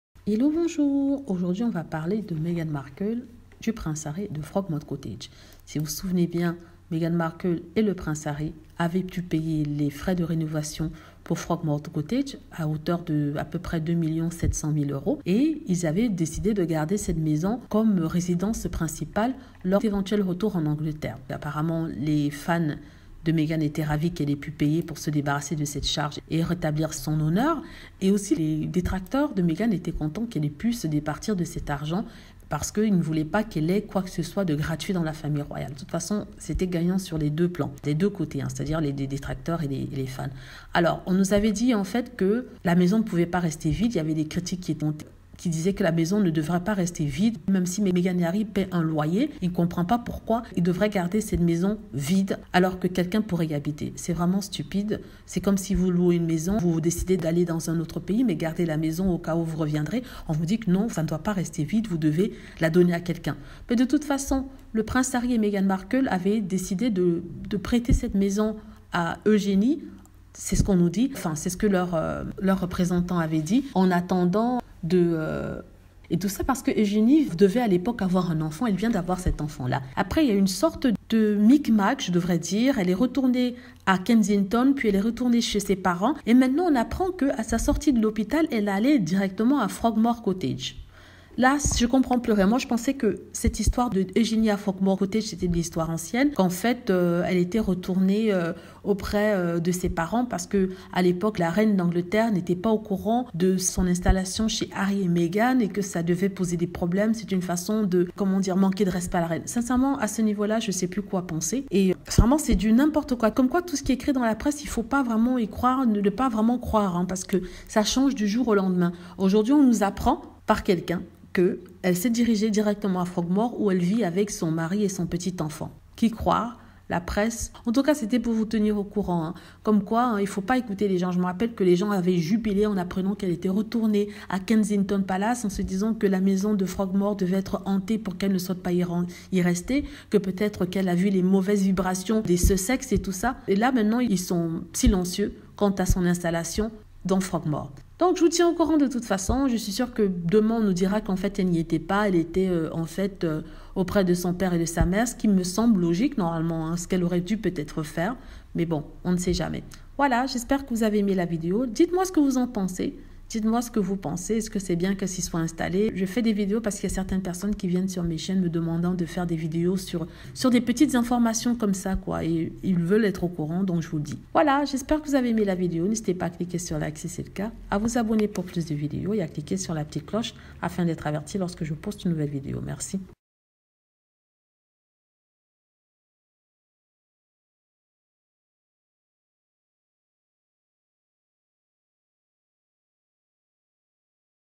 La Princesse Eugenie de nouveau à Frogmore Cottage chez Meghan Markle et Harry.
la princesse eugenie a finalement décidé de s'installer chez le Prince Harry et Meghan Meghan Markle à sa sortie de la maternité. La cousine du Prince Harry vient juste d'avoir son bébé et avait besoin d'un endroit ou résider, sa résidence de Kensington palace étant trop petite.